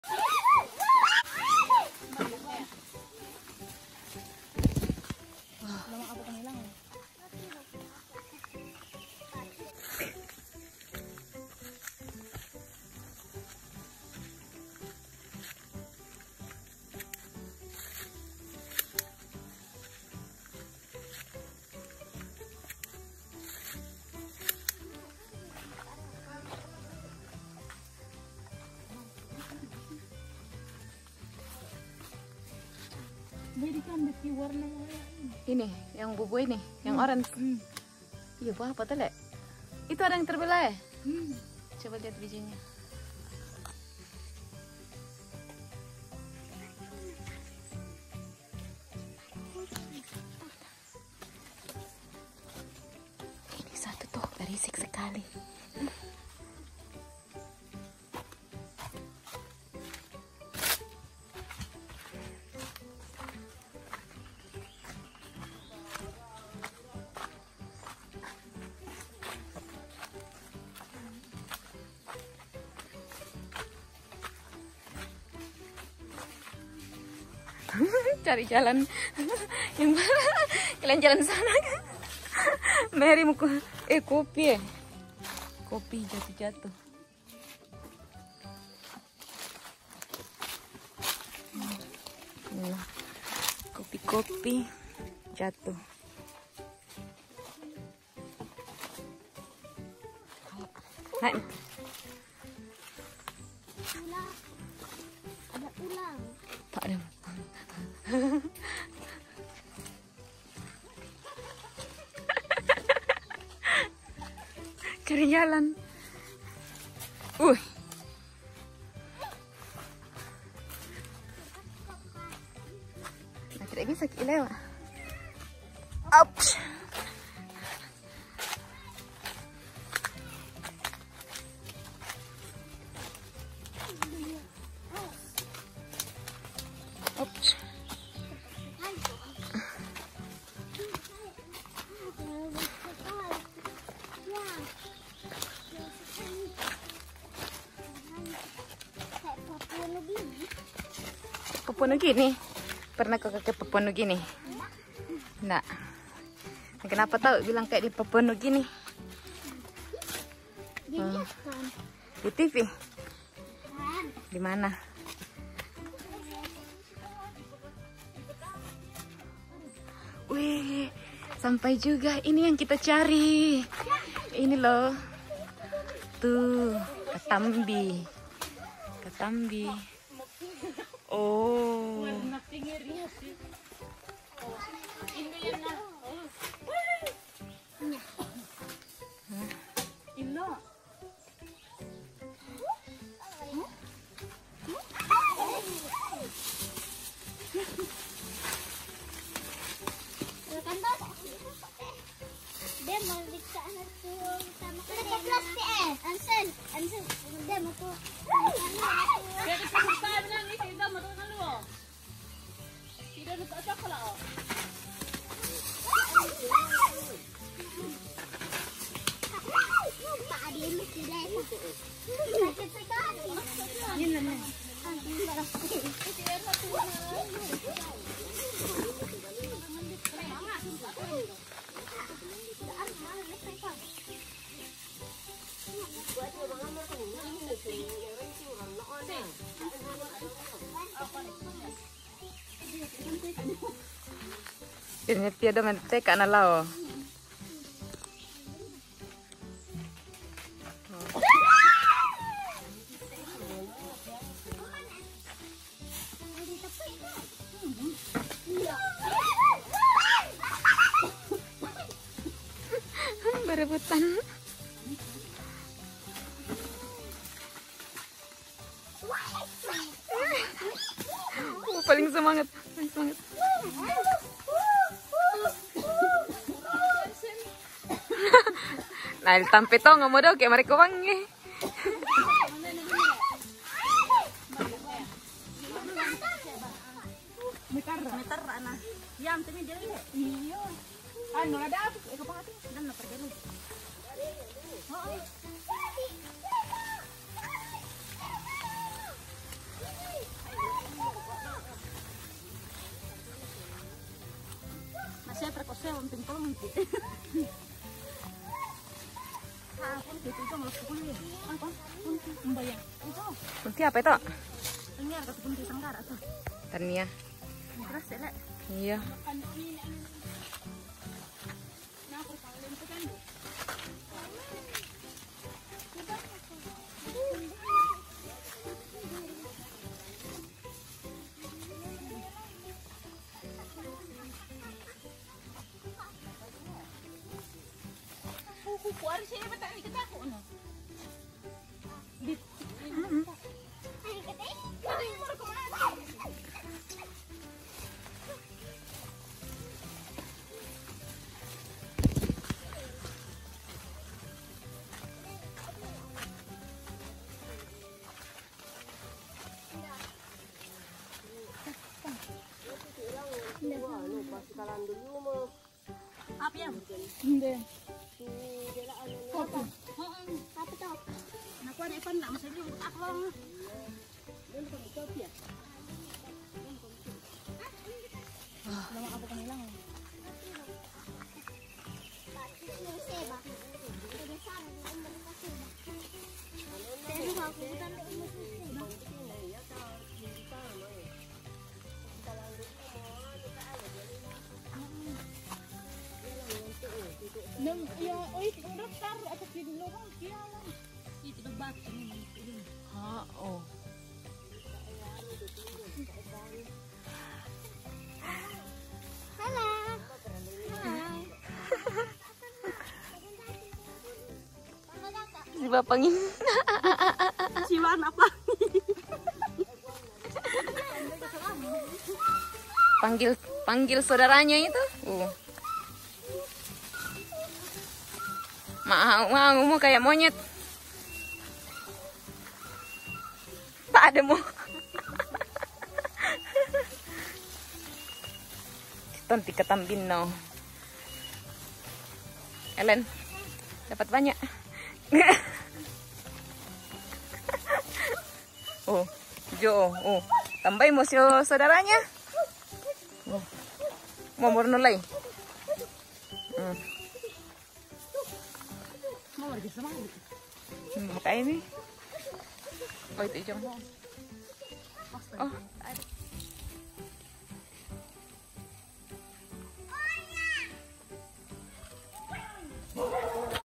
KATAMBI - kehutan cari buah ini, pas nemu girangnya ngalahin suara Monyet Sarira
Hidup di desa, Bahagia itu sederhana, masuk hutan cari buah kesukaan saja sudah senangnya luar biasa..